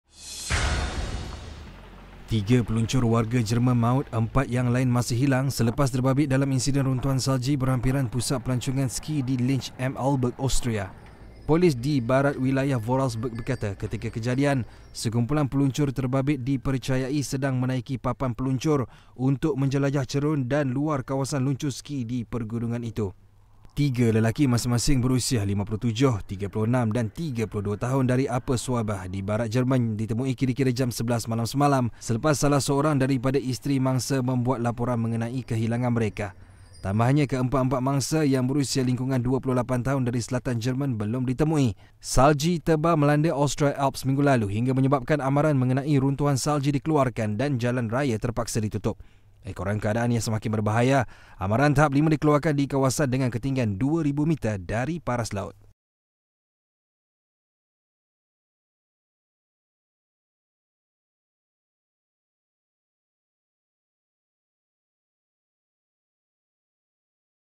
3 Peluncur Maut
Tiga peluncur warga Jerman maut, empat yang lain masih hilang dalam runtuhan salji.